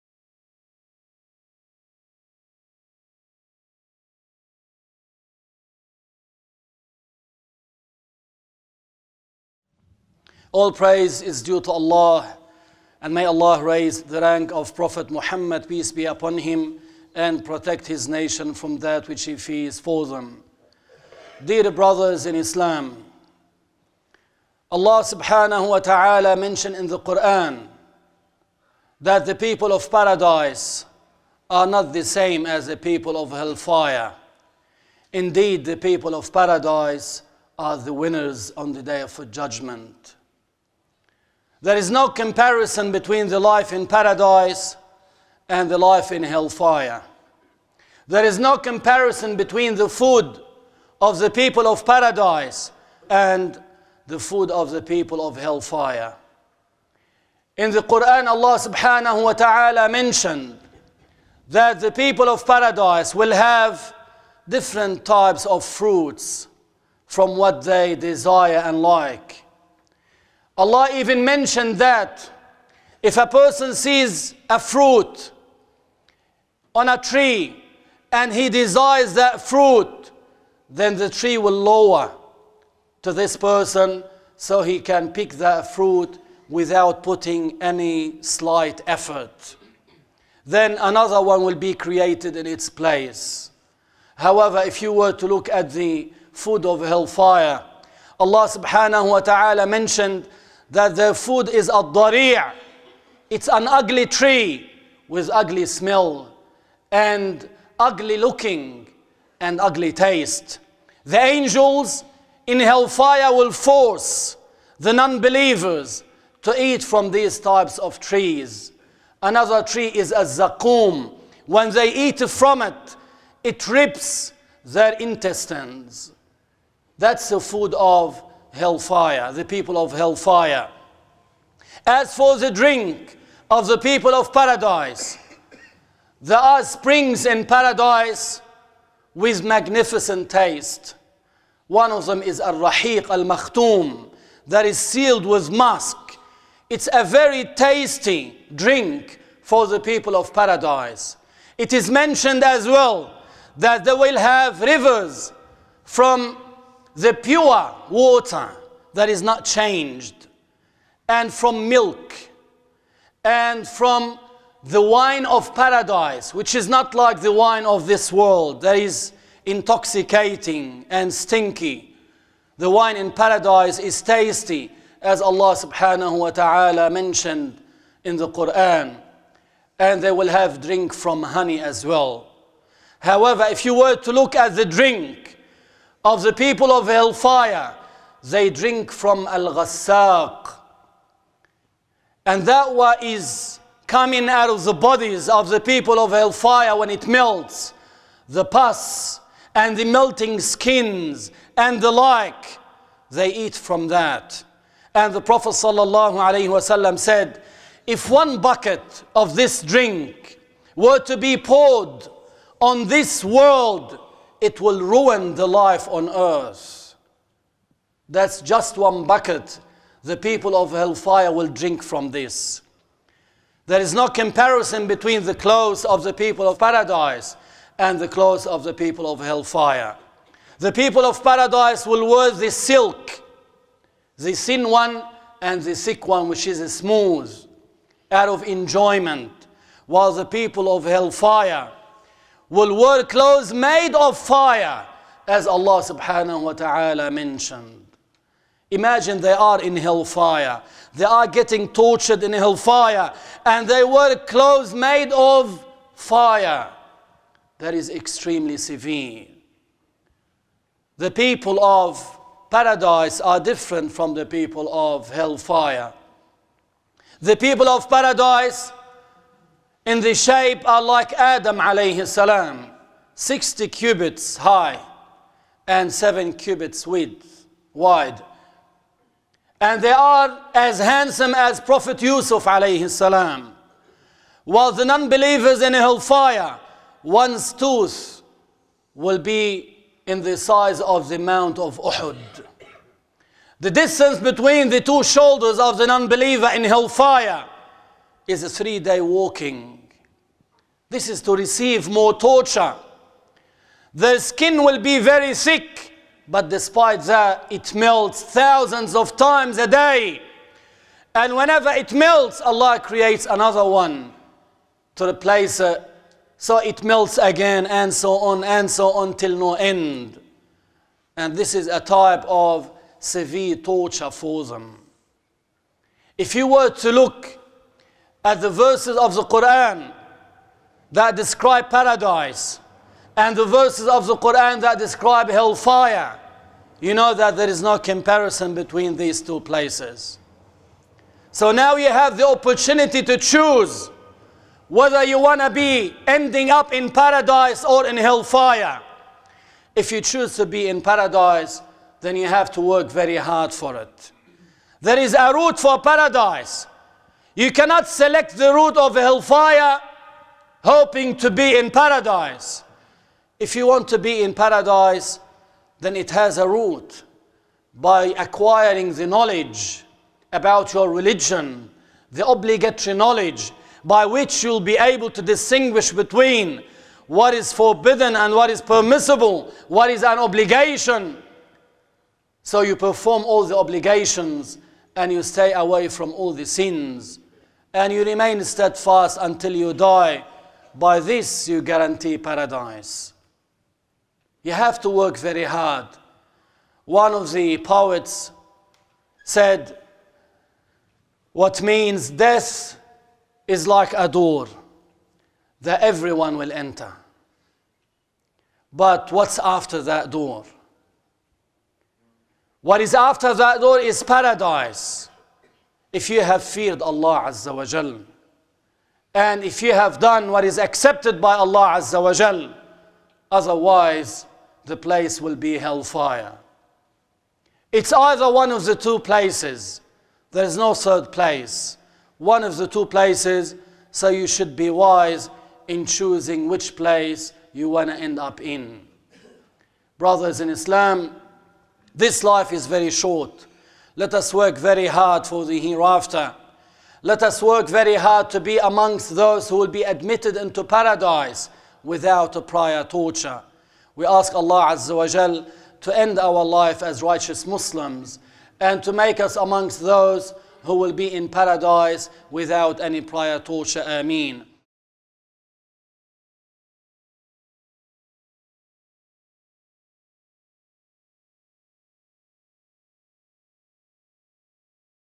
The Everlasting Hereafter - Sheikh Ibrahim El-Shafie
Allah mentioned in the Qur’an that the people of Paradise are not the same as the people of Hellfire – Indeed the people of Paradise are the winners on the day of judgement. There is no comparison between the life in Paradise and the life in Hellfire. There is no comparison between the food of the people of Paradise and the food of the people of hellfire.  In this Sermon sheikh briefly talks about the life in Paradise and life in Hellfire.